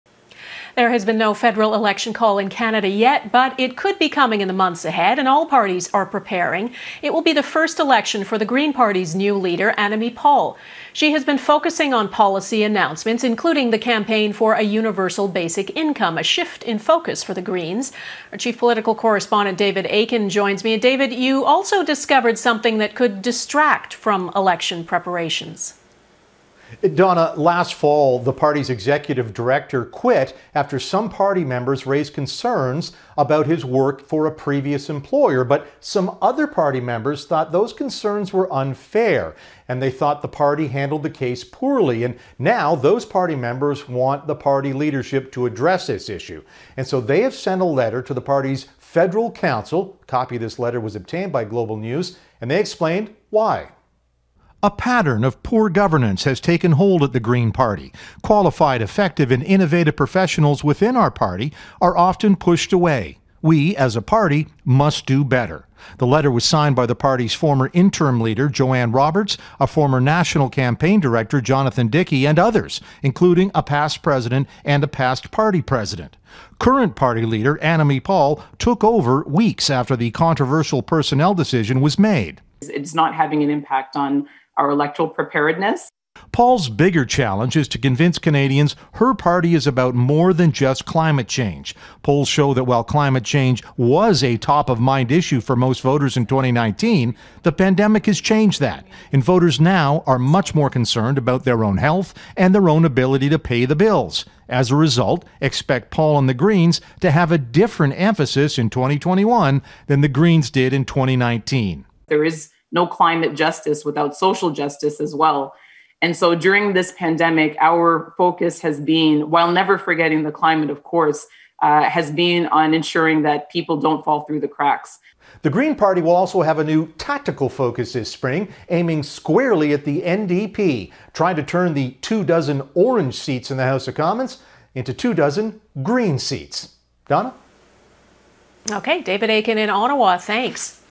Green Party takes aim at NDP seats with new strategic focus
Green Party Leader Annamie Paul has been prepping policy announcements and discussions as the party gears up for a possible spring election, including one Monday that reaffirmed the party’s position to campaign in favour of a universal basic income.

And within days, the party will officially announce its new national campaign director, an individual that, party sources say will be tasked with stealing votes that went to Jagmeet Singh’s New Democratic Party in the 2019 general election. The party wants to turn the two dozen orange seats in the House of Commons into two dozen green seats.

But even as it prepares for the potential of Canadians going to the polls, the Green Party of Canada is being forced to deal with a controversial internal personnel decision that some party members say could be a distraction that could hurt the party’s election preparations. David Akin has more.